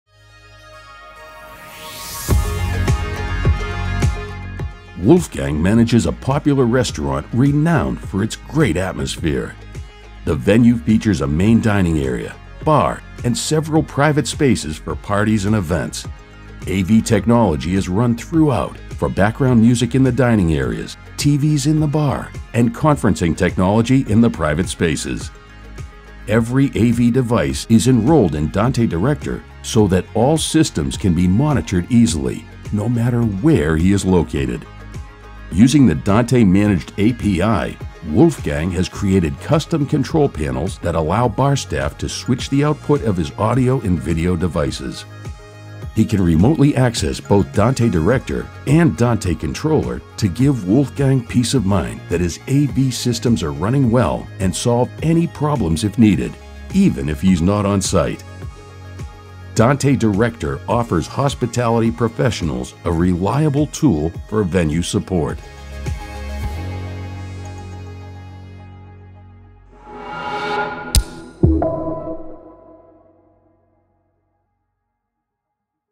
Dante Director for Restaurants, Bars, and Clubs
Busy restaurants, bars and clubs utilize a wide array of audio and video technology that can require frequent reconfiguration across regions and zones. The Dante Managed API, included with Dante Director subscription, enables the creation of simple interfaces to manage complex systems.  Dante Director gives restaurants, bars and clubs new ways to manage and monitor devices that are integral the business.

Try Dante Director with your network with a free 30-day trial